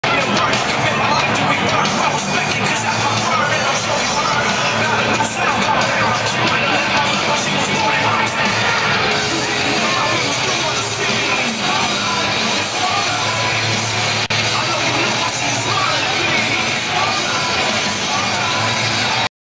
My 1st Patriots Game 4 of 4
Pats vs. Jets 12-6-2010 Teddy Bruschi night.